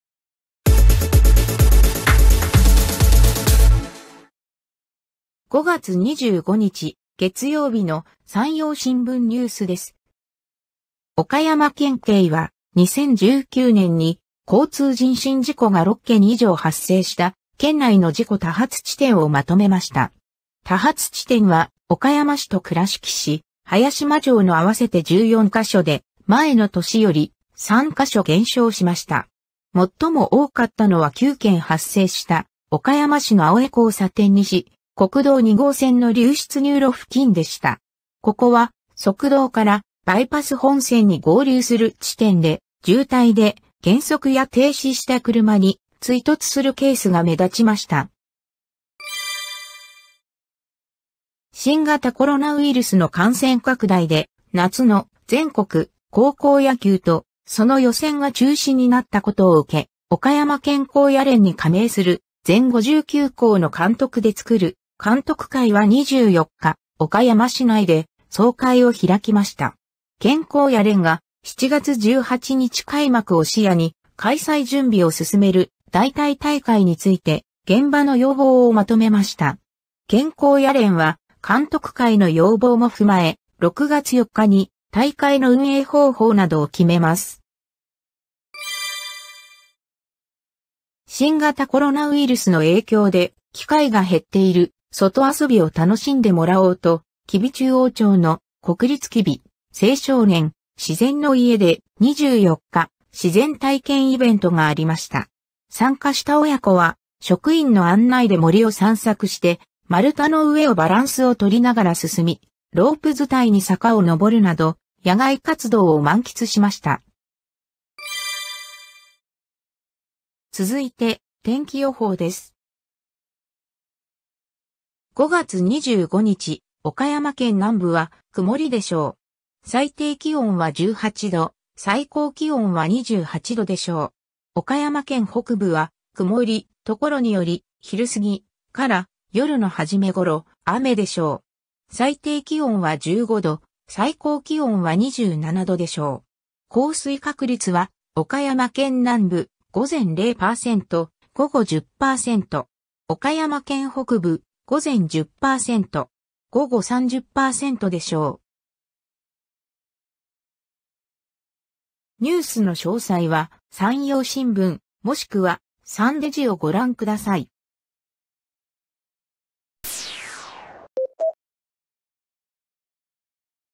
【５月２５日】県内の交通人身事故多発地点は ＡＩアナＮＥＷＳ
県内の交通人身事故多発地点は
高校野球代替大会要望まとめる
吉備中央で自然体験イベント